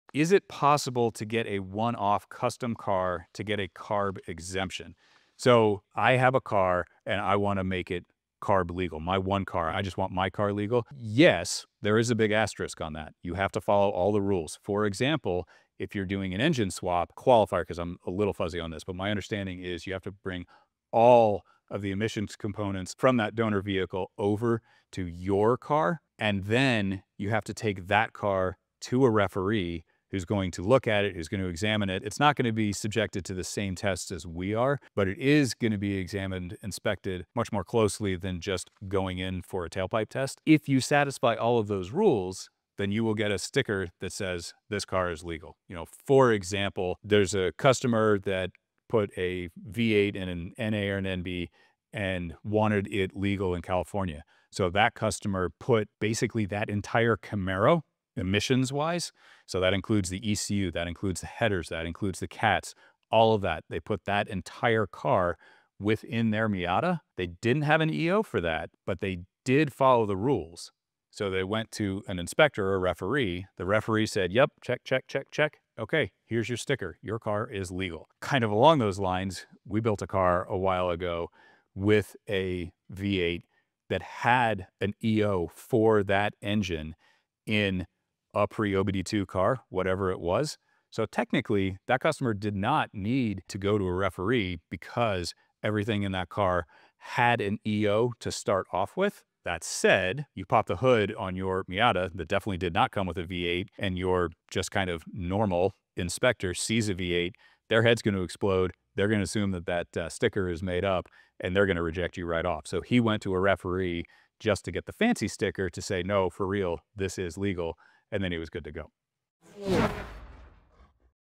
CARB Exemption How to Make Your Custom Car Legal!
This week on FM Live, our Director of Product Development, Brandon Fitch, shares insight into emissions regulations and how they affect us here at Flyin’ Miata. What is CARB, how does it work, and what does it mean for the Miata community?

 Miata is the world leader in Miata performance. We offer a wide range of parts for all years of the Mazda MX-5 Miata.